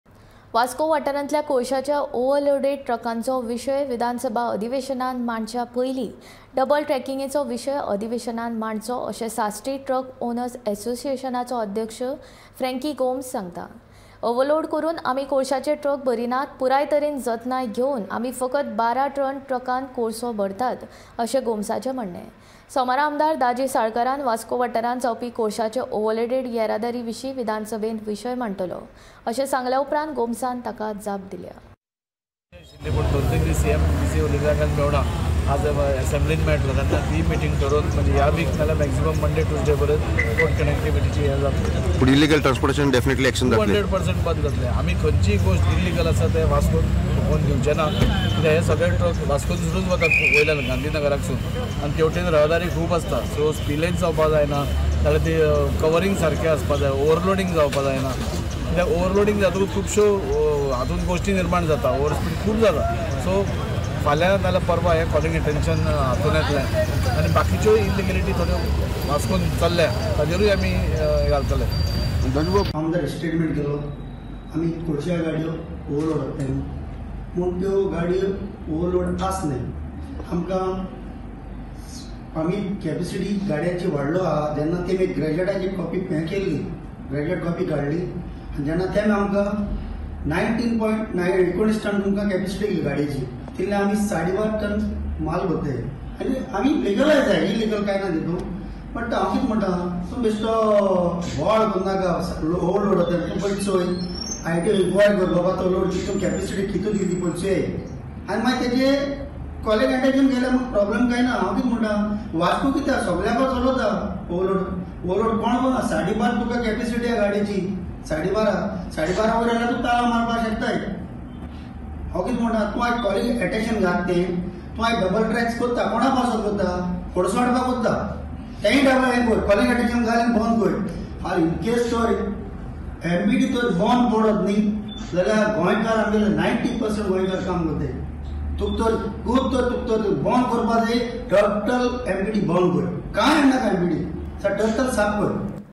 ओव्हरलोड ट्रकच्या मुद्द्यावर ट्रकमालकांचे वास्कोच्या आमदारांना आव्हान | Gomantak Tv
ओव्हरलोड ट्रकच्या मुद्द्यावर ट्रकमालकांचे वास्कोच्या आमदारांना आव्हान | Truck owners challenge vasco MLA on overloaded trucks issue | Gomantak Tv